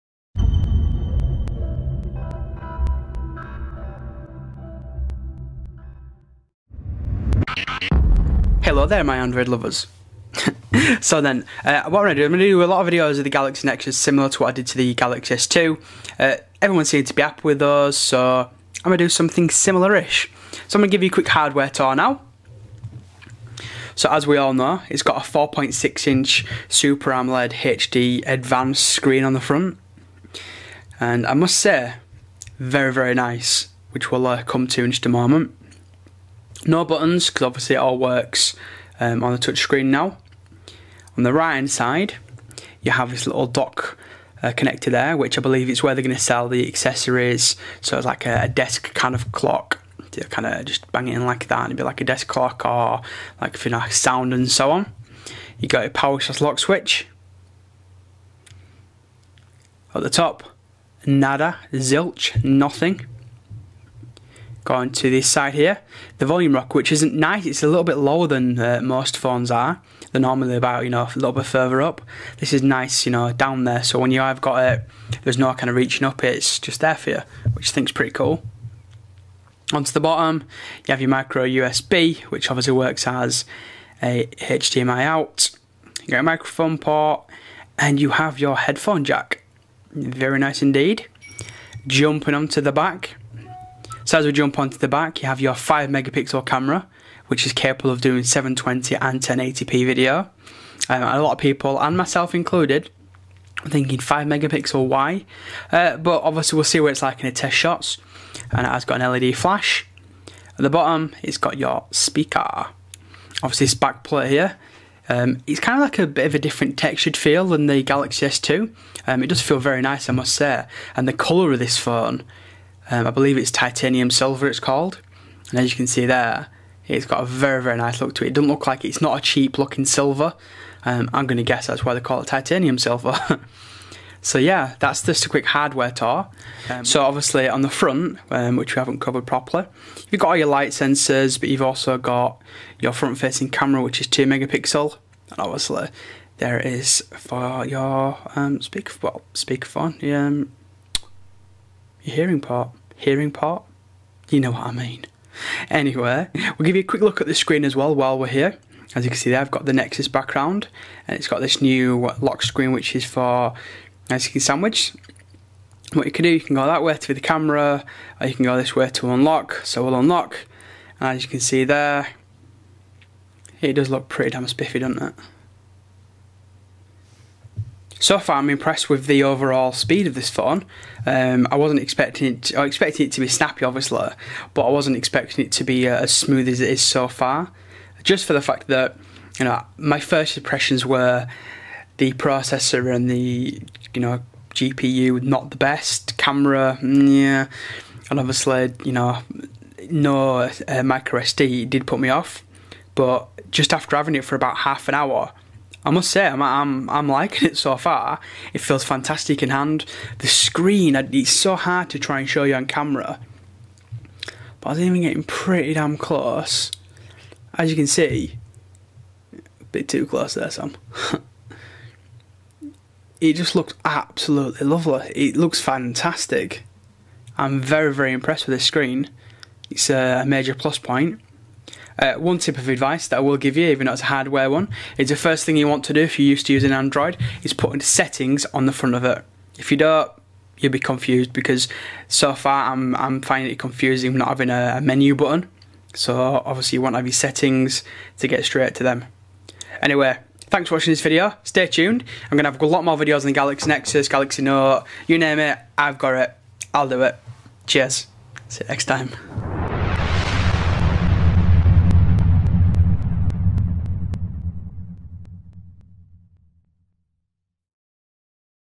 Galaxy Nexus Hardware Tour - SAMJPULLEN
More detailed Hardware Tour of the new Galaxy Nexus from Samsung/Google.